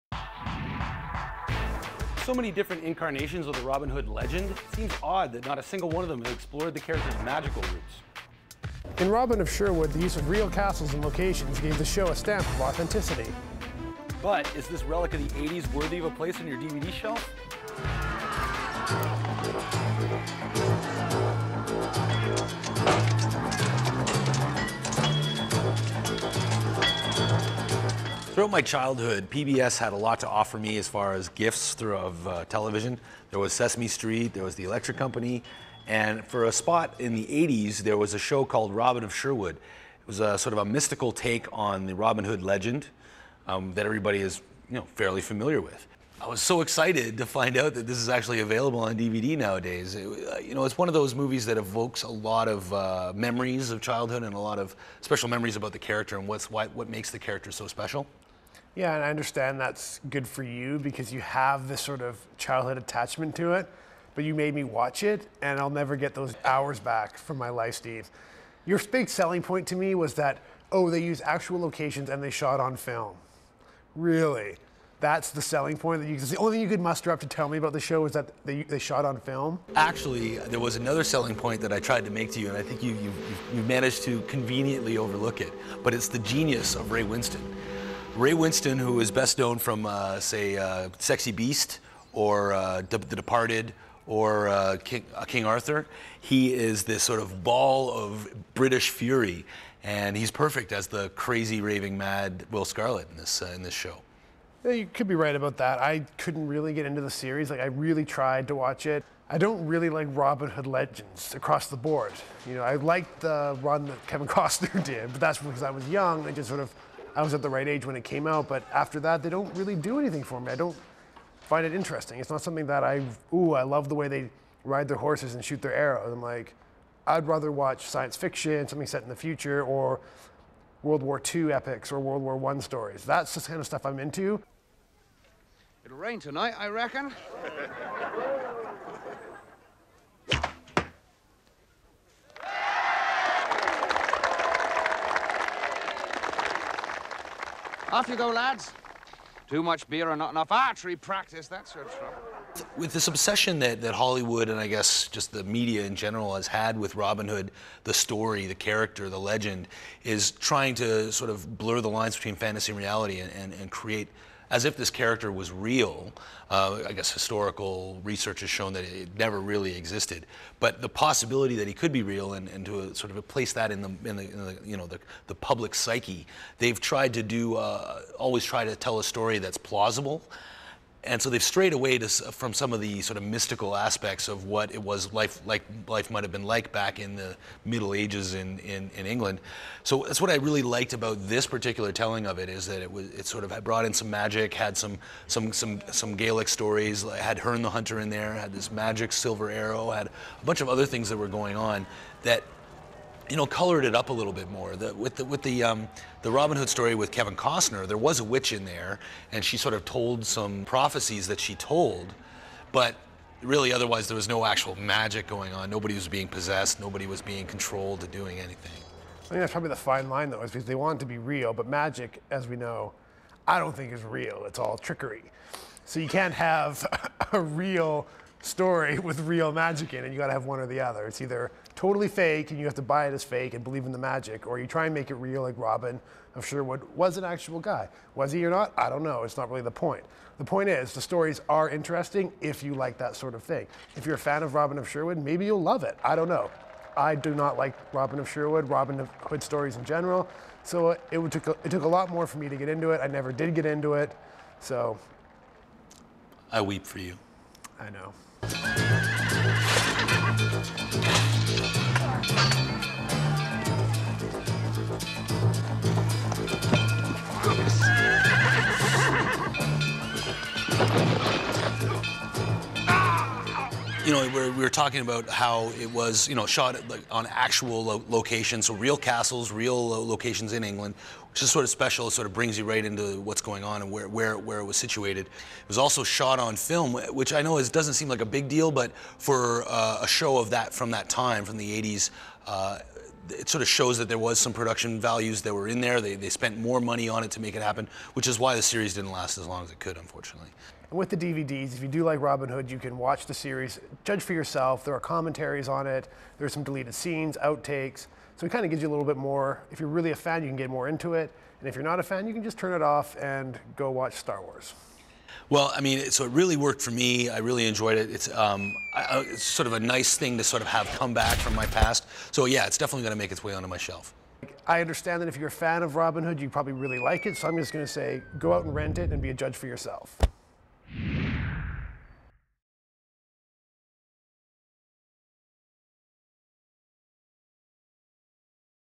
The Film Cynics - Robin of Sherwood
It's never been clear why adaptations of the Robin Hood legend have never explored a magical side of his nature. I suppose the studios have figured that keeping things in the mundane, makes this fictional story more believable. Robin of Sherwood manages to infuse the legend with magic and wonder, and also makes excellent use of real locations.